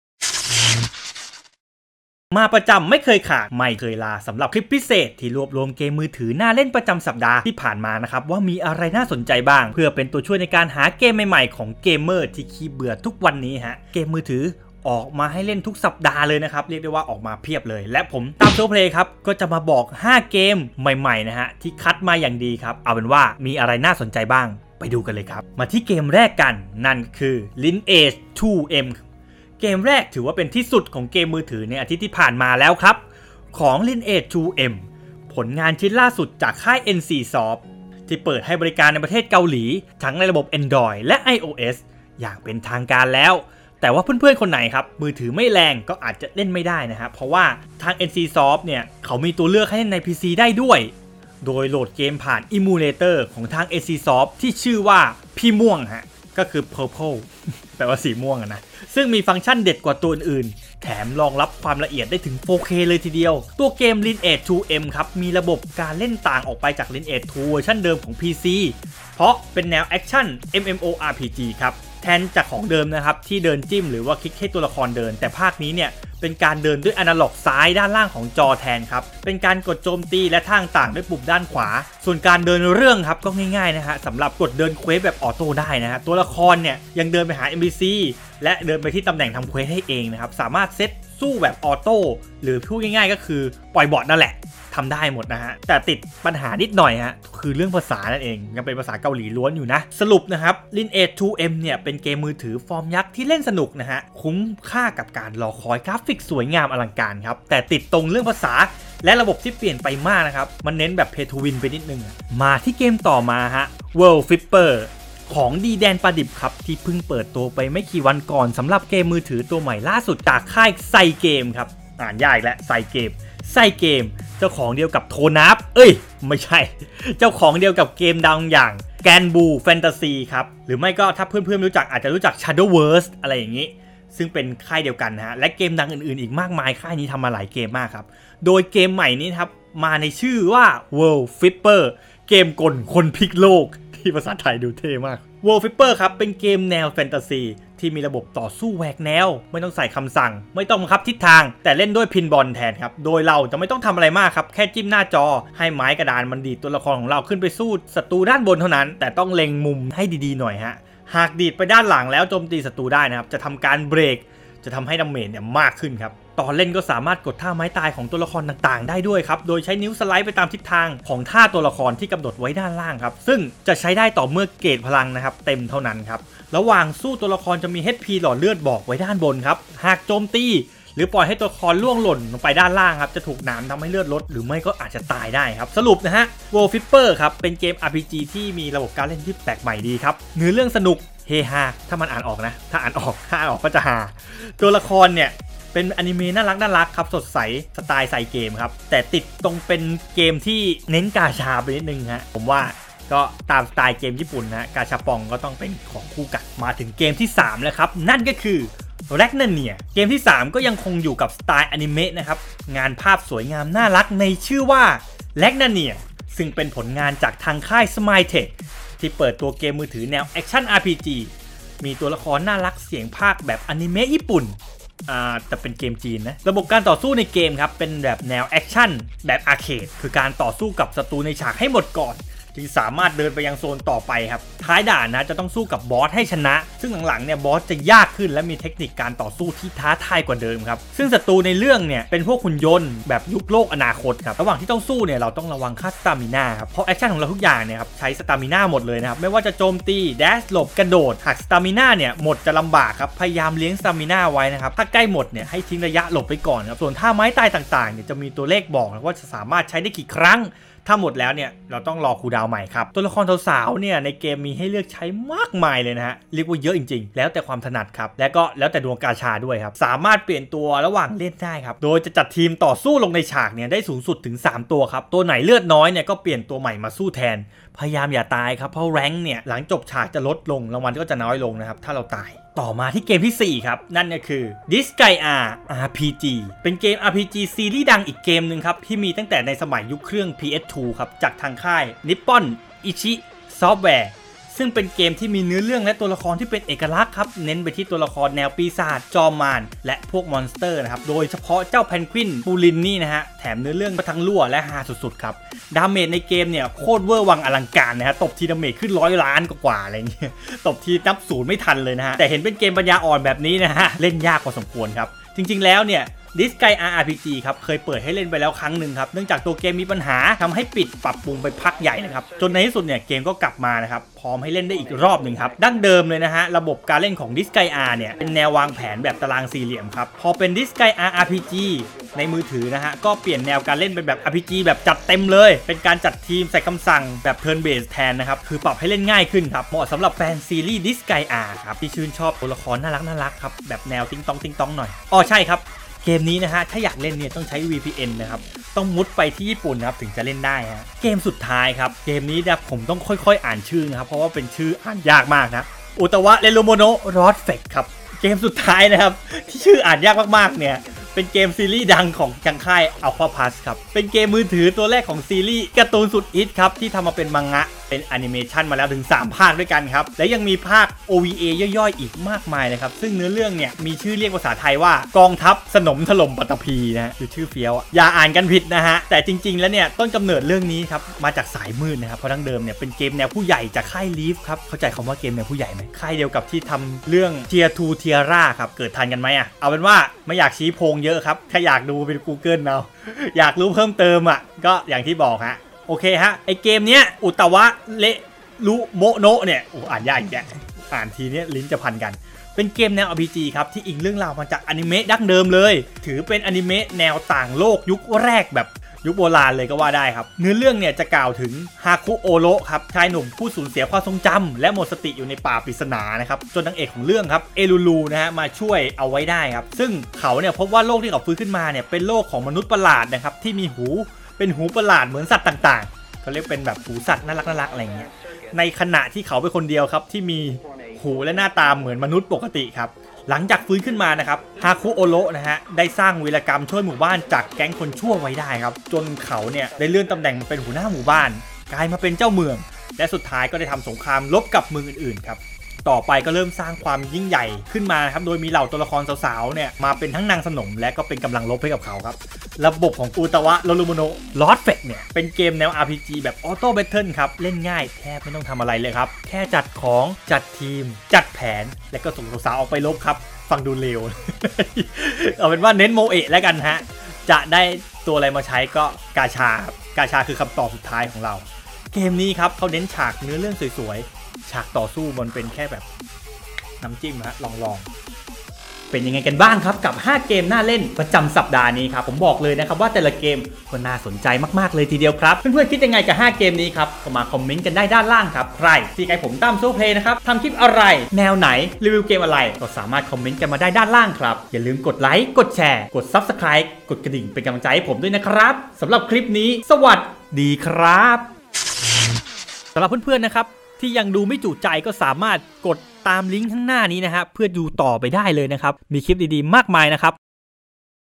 5 เกมน่าเล่นประจำสัปดาห์ EP:3 - เกมใหม่ๆที่เพื่อนๆไม่ควรพลาด!
มาประจำทุกสัปดาห์ไม่เคยขาดไม่เคยลาสำหรับบทความพิเศษที่รวบรวมเกมมือถือน่าเล่นประจำสัปดาห์ที่ผ่านมาว่ามีอะไรน่าสนใจบ้างเพื่อเป็นตัวช่วยในการหาเกมใหม่ๆ เล่นของเกมเมอร์ขี้เบื่อที่ทุกวันนี้เกมโทรศัพท์มือถืออกกันให้เลือกทุกอาทิตย์เลย แน่นอนว่า 5 เกมที่คัดมาก็ถือว่าน่าสนใจทั้งนั้นจะมีอะไรบ้างไปชมกันเลย

ติดตามข่าวสารของพวกเราได้ที่